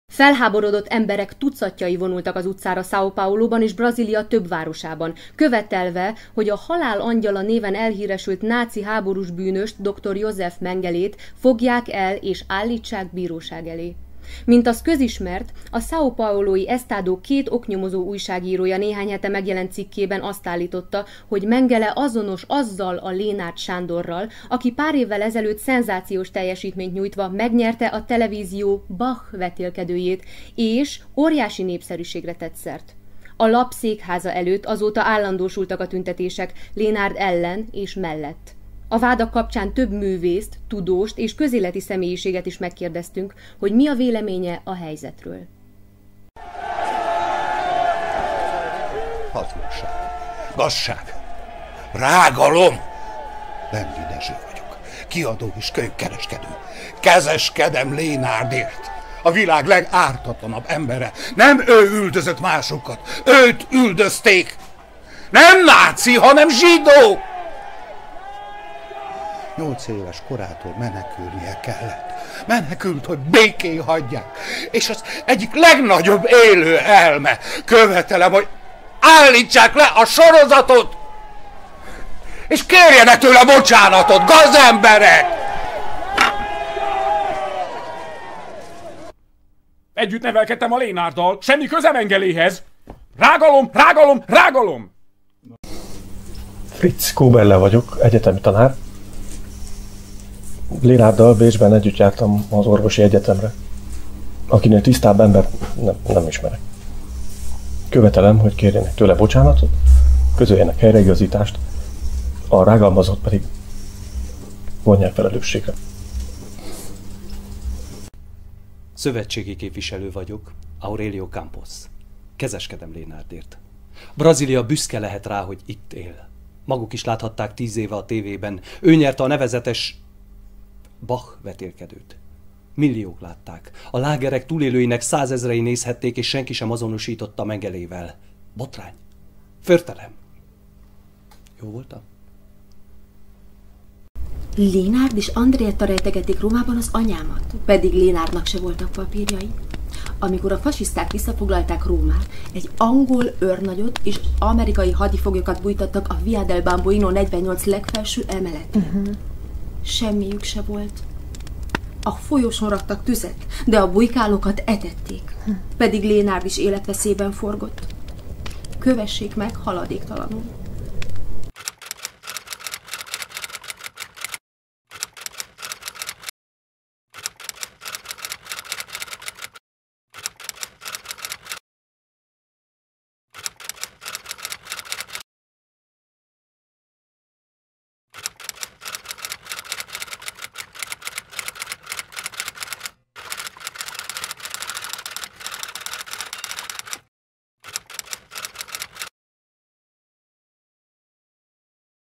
Helló, doktor Mengele! - videóajánló
Spiró György:
Helló, doktor Mengele!
ŐSBEMUTATÓ

Szereposztás:
Lénárd Sándor: Rappert-Vencz Gábor
Rabinovitz újságíró: Rappert-Vencz Gábor
Báró Korányi Janó volt kisgazda, igazgatósági tag: Kányádi Szilárd
Paulo Cruz Pimentel kormányzó: Kányádi Szilárd
Tapsos ember: Kányádi Szilárd
Báró Korányi Erzsébet: Kovács Nikolett
Báró Korányi Janó nővére: Kovács Nikolett
Szerb Klára irodalomtörténész: Kovács Nikolett
Senhor Tiberio lottóárus, szentképfestő: Péter Attila Zsolt
Margitka, Senhor Tiberio felesége: Sándor Anna
Utas: Sándor Anna Rappert-Vencz Stella
Beosztott tiszt: Sándor Anna
Landy Dezső kiadó: Bessenyei István
Beöthy Ottó, a Világszövetség főtitkára: Bessenyei István
Piloto rendőrfőnök: Bessenyei István
Hamza D. Ákos filmrendező: Nagy Orbán
Rafi Eitan titkosügynök: Nagy Orbán
Őrmester: Nagy Orbán
Kardos György kiadóigazgató: Nagy Orbán
Hamzáné Lehel Mária: Rappert-Vencz Stella
Tulio műsorvezető: Rappert-Vencz Stella Poszet Nándor
Vedernjak Franjo nyomdász: Poszet Nándor
Ügynök: Poszet Nándor
Isser Harel, a Moszad vezetője: Varga Sándor
Fernando Camargo, újságíró: Varga Sándor
Főszerkesztő: Varga Sándor
Kameraman: Varga Sándor Gaál Gyula
Vedernjak Stefan nyomdász: Gaál Gyula
Dr. John Asa Hamrick tiszteletes: Gaál Gyula
Erich Erdstein kávéárus: Bodea Tibor
Kábeles technikus: Bodea Tibor
Kleinné: Méhes Kati
Fritz Kuberle: Kányádi Szilárd
Picher Károly szobrász: Péter Attila Zsolt
Aurelio Campos képviselő: Nagy Orbán
Rendező:  Lendvai Zoltán m.v.
Díszlettervező:  Fornvald Gréti m.v.
Jelmeztervező:  Szabó Anna
Súgó:  Varga Katalin
Ügyelő:  Varga Katalin
Asszisztens:  Tamás Ágnes
Bemutató: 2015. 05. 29. Szakszervezetek Művelődési Háza, Szatmárnémeti, nagyterem, színpadi nézőtér

Előadás-ismertető:

Lénárd Sándor (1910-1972) sokoldalú egyéniség, hihetetlen képességű férfi, egy modernkori polihisztor volt. A teljes európai kultúra birtoklója, orvos, író, zenész, műfordító, költő és nyelvzseni egy személyben. A Második Világháború idején a fasizmus elől Brazíliába menekül. Az emigrációban több fontos munkája mellett latin nyelvre fordította a Micimackót, és hatalmas összeget nyert egy televíziós Bach-vetélkedőn.
Ezt a felbecsülhetetlen szellemi értéket képviselő gondolkodót, a hatvanas évek elején, azzal a képtelen váddal illették, hogy ő doktor Mengele, az auschwitzi haláltábor hírhedt orvosa. Spiró György ezt a pokoli, ám valóságos élettörténetet sötét komédiában fogalmazta meg, amely valamennyi szereplőjét valós személy ihlette.